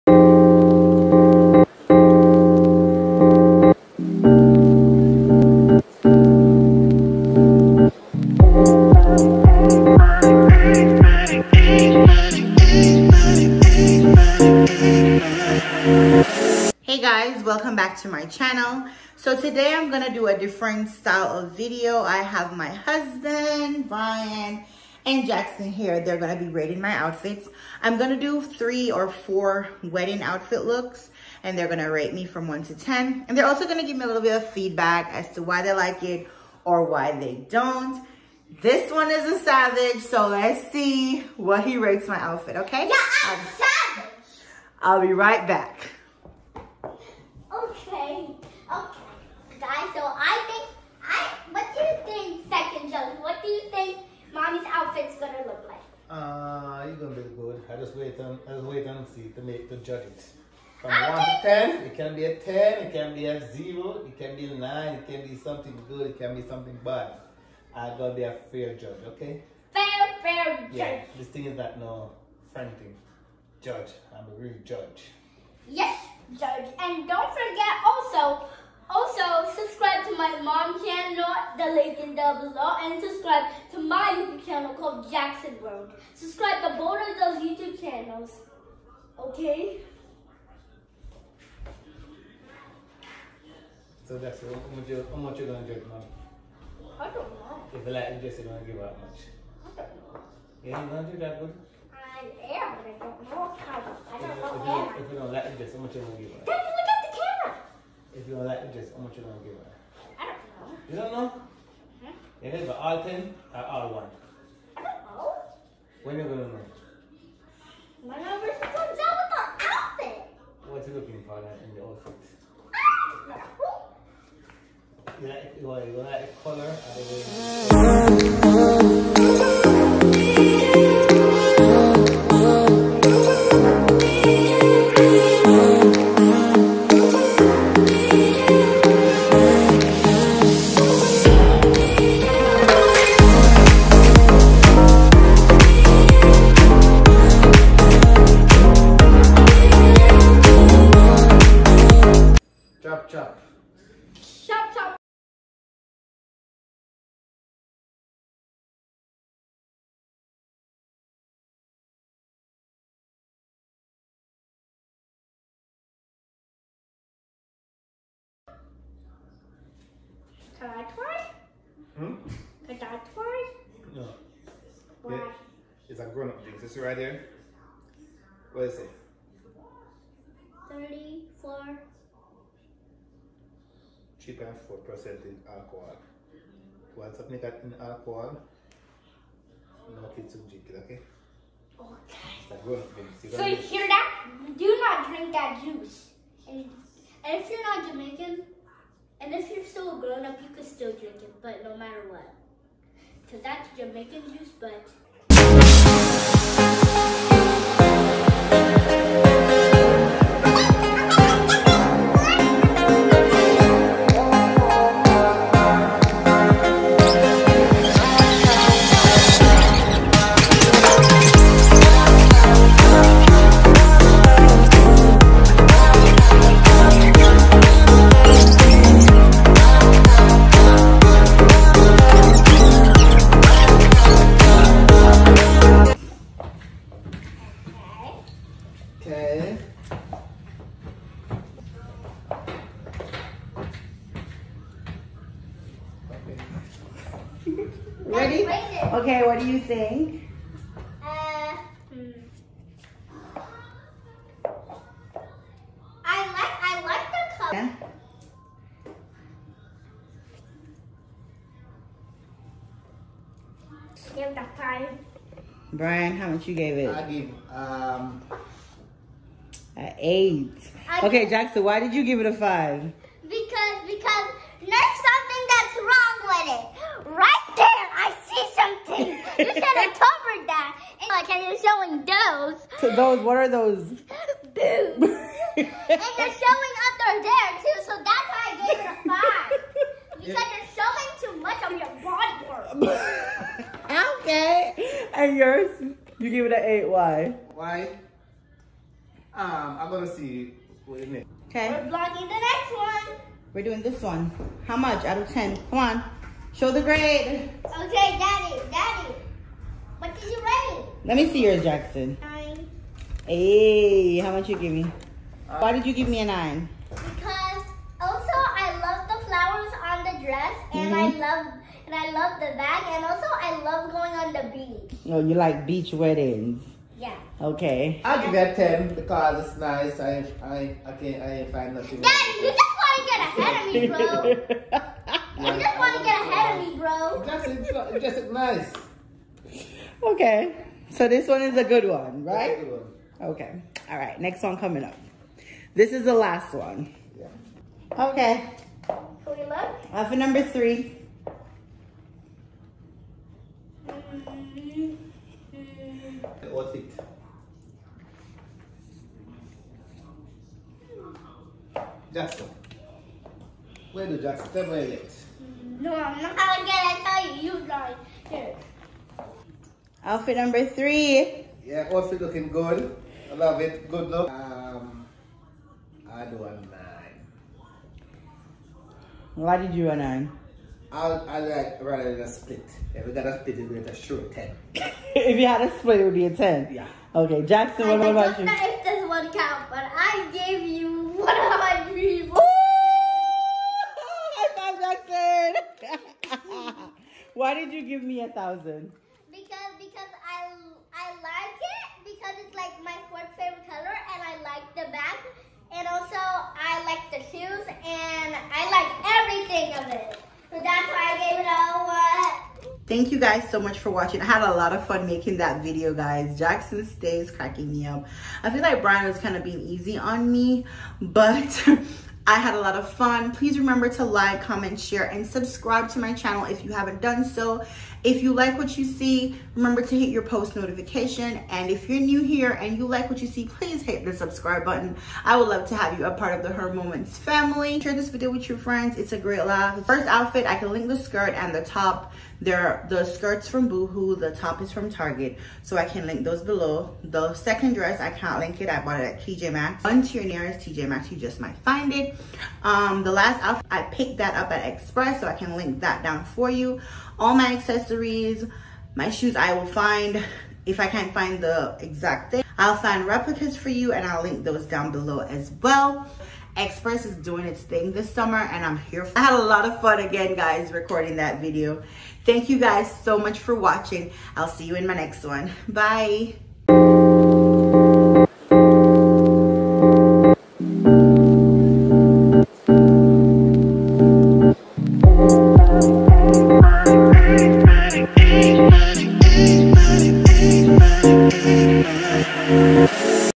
Summer Wedding Dresses (Hubby & Son rate my outfit)
Hey Beauties I picked up a few dresses for Wedding Season, let me know which is your fave!

Pink Skirt
Pink Bralette
Black Dress
Link Here ⬇️